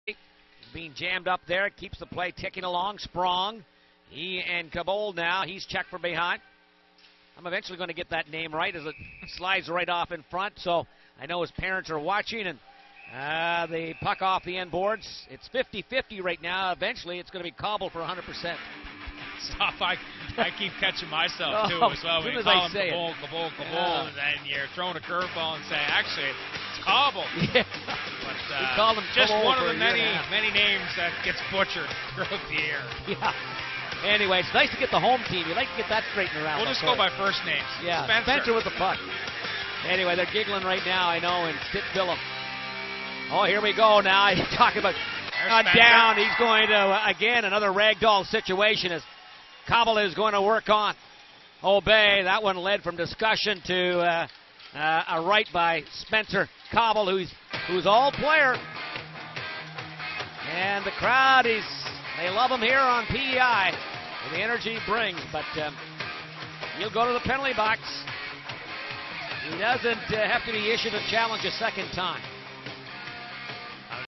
Frederic Aube vs Spenser Cobbold Nov 16, 2014
Frederic Aube vs Spenser Cobbold from the Drummondville Voltigeurs at Charlottetown Islanders game on Nov 16, 2014.